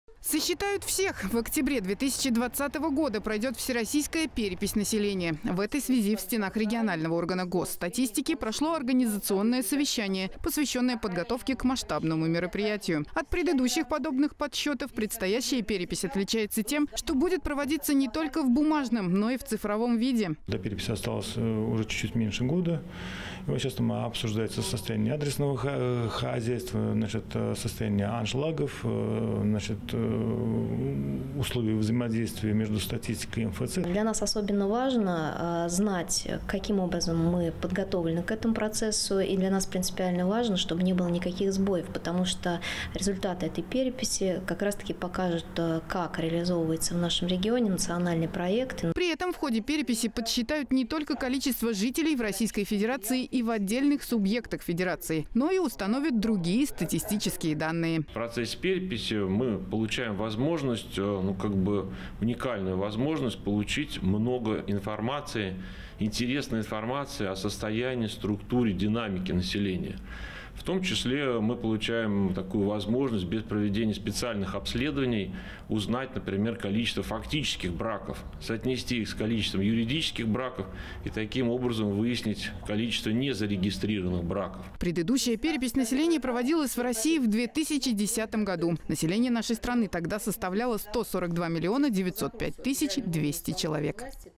Сосчитают всех
В октябре 2020-го года пройдёт Всероссийская перепись населения.